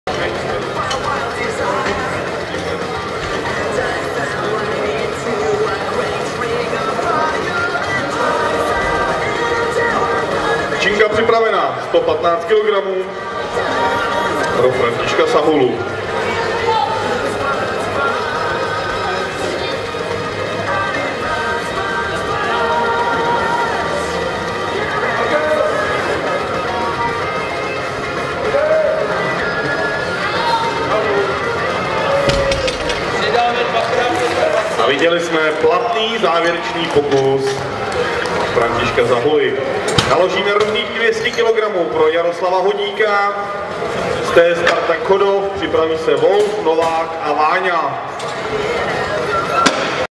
Sahula František (1965), MT3, 115 kg
3. Otevřené Mistrovství ČR v klasickém (RAW) silovém trojboji masters

Místo: Sportovní hala, Praha - Chodov